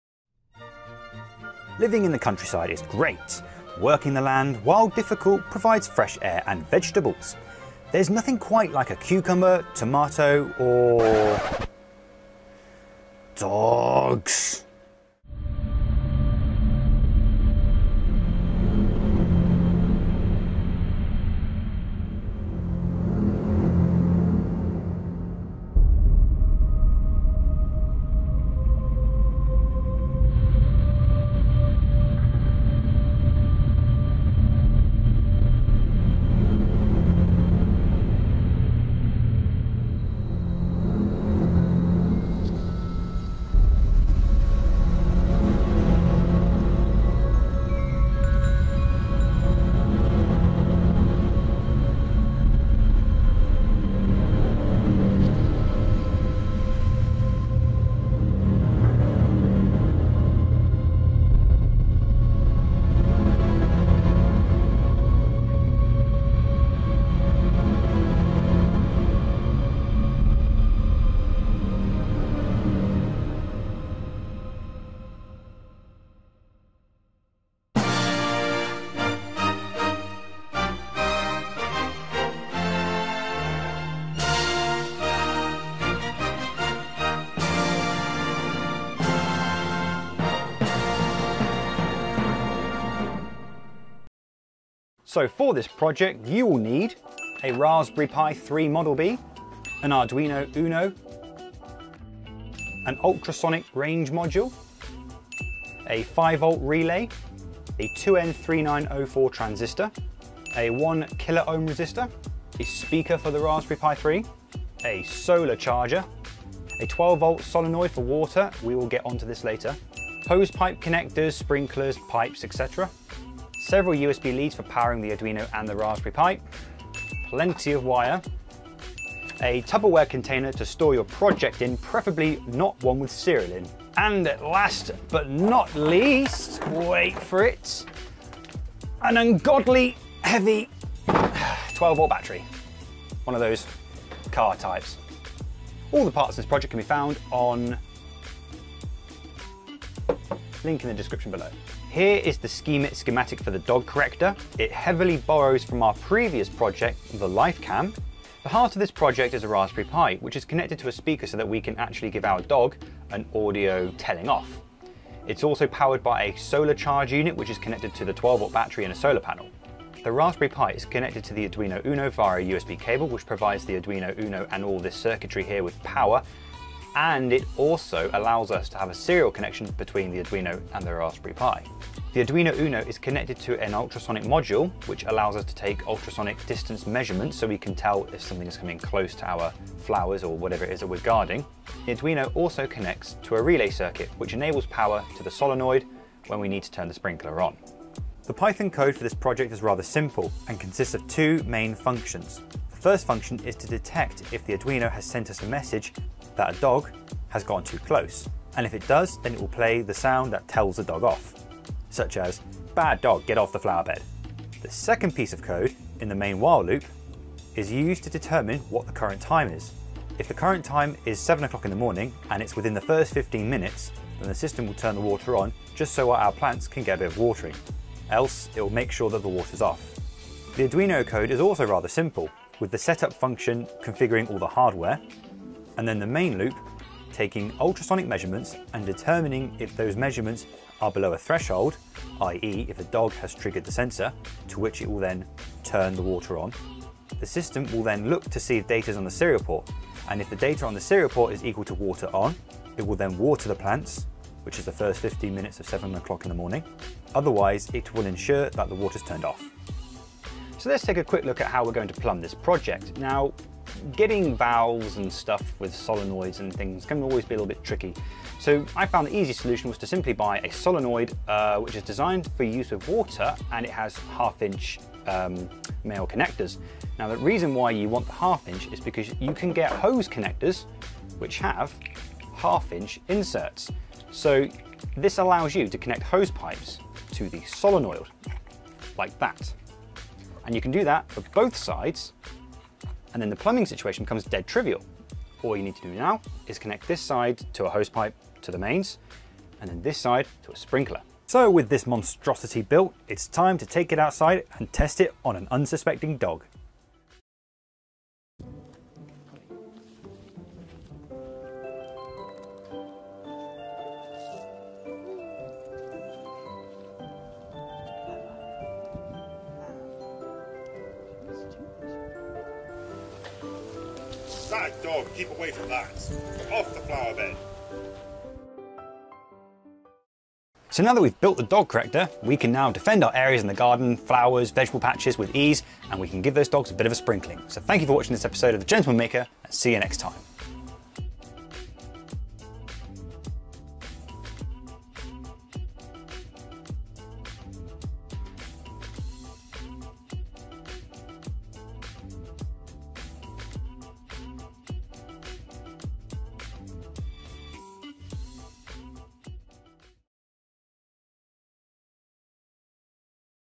How to Build Your Own Dog Corrector
In this episode of the Gentleman Maker, we're using a Raspberry Pi and an Arduino Uno to stave off the onslaught of dogs trying to eat every single plant in our garden.

Check out more video topics and articles coming from Robin Mitchell

Dog Corrector BOM

Speaker
Solar panel
Solar charger controller
12V lead-acid battery
Waterproof box
Wires and circuit construction materials